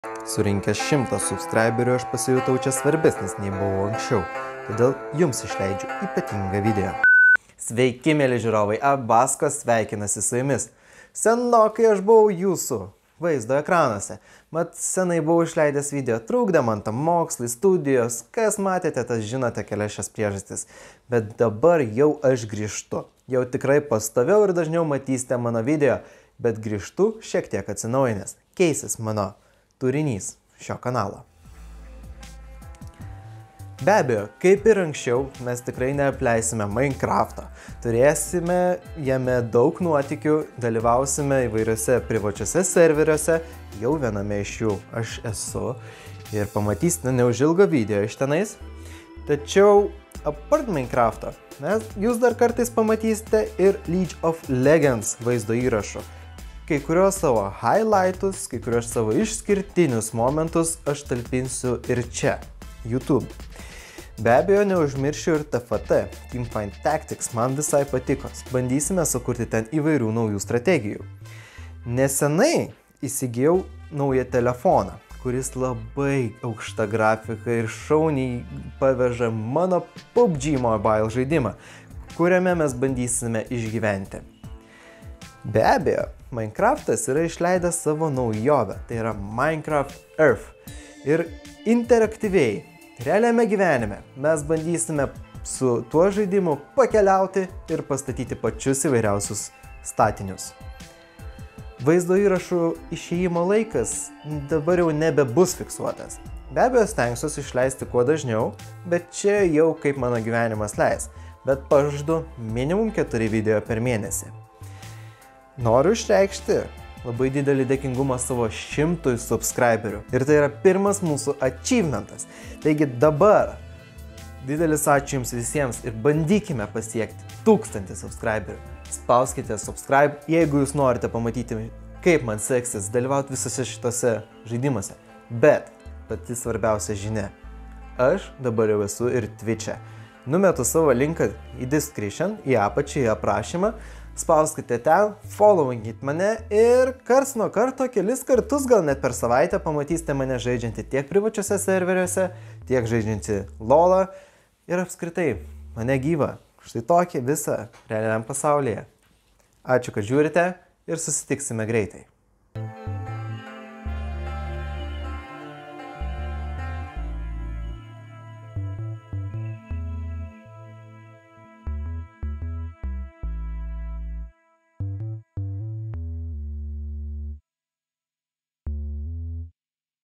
Aš grįžtu! 🙌 100 Prenumeratorių 🎈Special video - KAS BUS MANO KANALE TOLIAU? 🎉
Ilgą laiką aš buvau dingęs iš Jūsų ekranų.
Tačiau aš pasiruošęs grįžti.

Taigi, noriu pranešti, kad mano kanale dabar matysite skirtingų žaidimų vaizdo įrašus ❗️
📌Minecraft. Kaip ir anksčiau, nusikelsiu į kubelių pasaulį, kuriuose keliausime, kažką atrasime, statysime ką tik norime ir patirsime nuotykių.
📌League of legends. Bandysime įveikti priešininkų komandas arba pasitelkti tam tikras taktikas TFT varžybose.
📌 PUBG mobile. Matysite įdomius nutikimus PUBGmobile žaidime, kuriame tikslas - išgyventi.
📌Minecraft earth. Ir išbandysime naujiena tapusiu žaidimu, kuris kvies mus išeiti į lauką, rinkti resursus ir ribotame žemės plote statyti kas tik į galvą šauna.

📅 Vaizdo įrašų išėjimo laikas nefiksuotas, video bus kuriami pagal asmenines galimybes, tačiau tikiuosi, kad ilgai laukti neužtruksite.

Na ir dar viena svarbi naujiena - mane galite rasti ir 💜TWICH💜 platformoje, kurioje kartais pamatysite mano streamus. Kviečiu juos stebėti, komentuoti ir bendrauti su manimi.